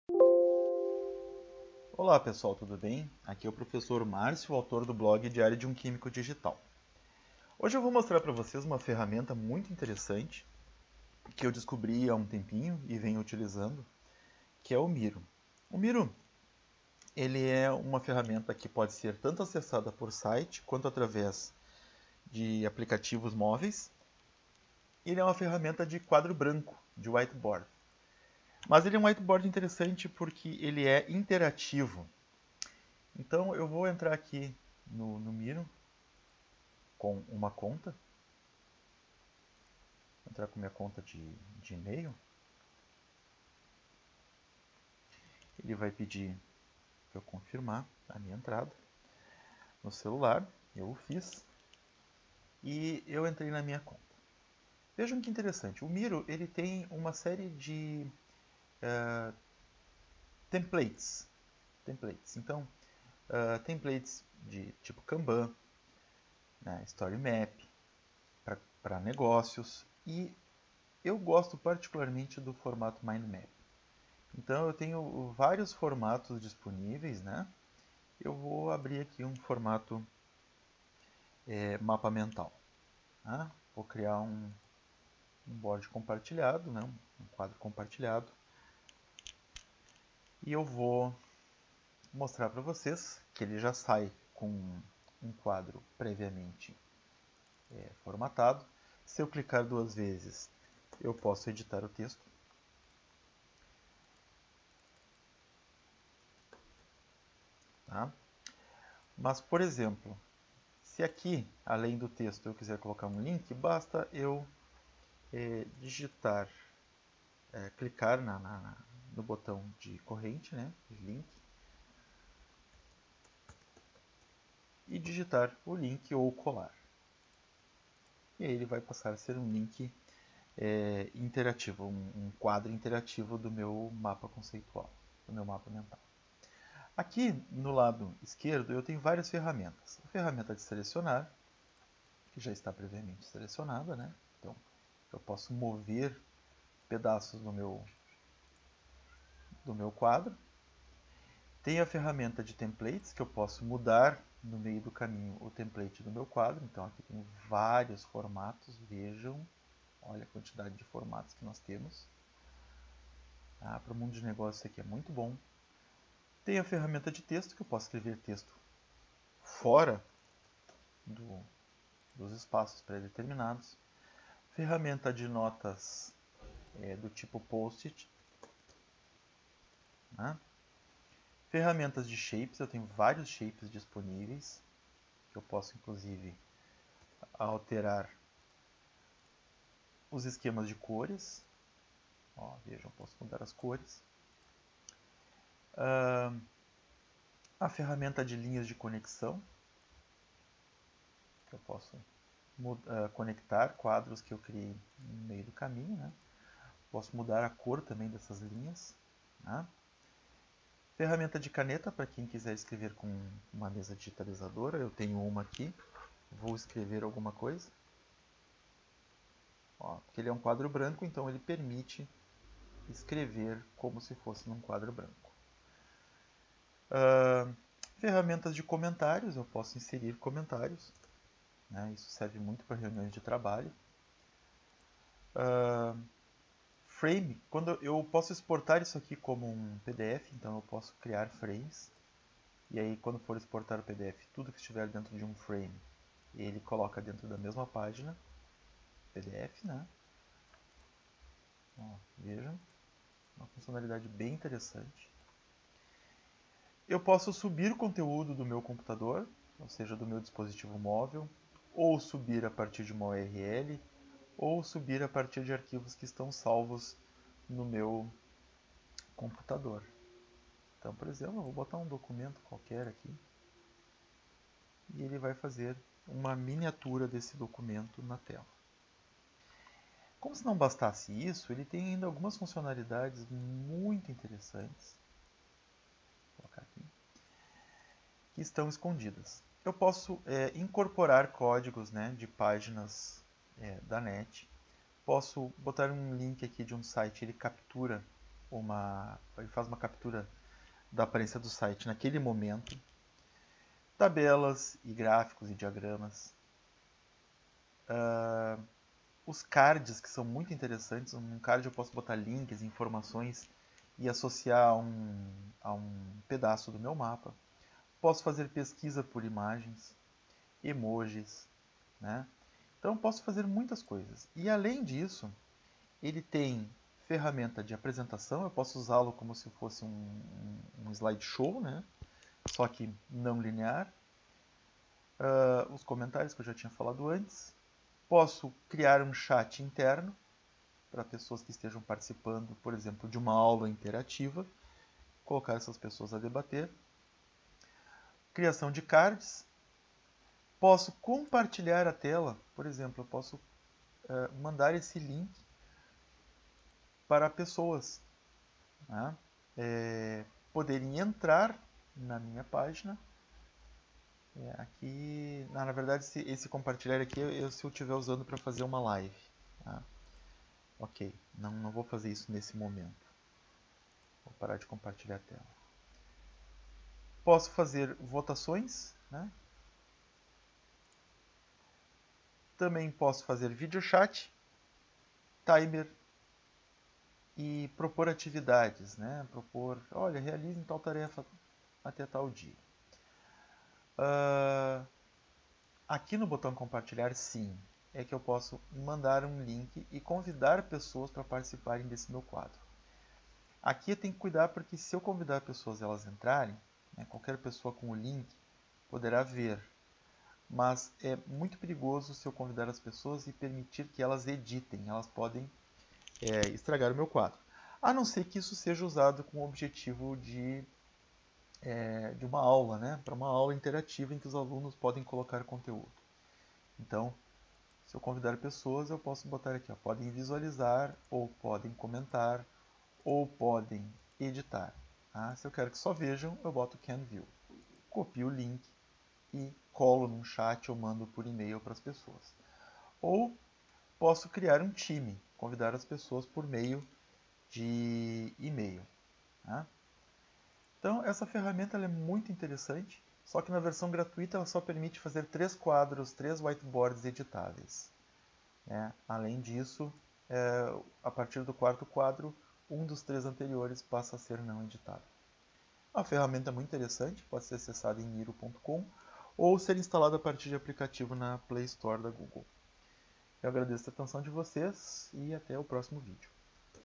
Apresentando o Miro
Ferramenta de Whiteboard colaborativo muito útil e versátil.
Acesse em miro.com ou instale o app via Play Store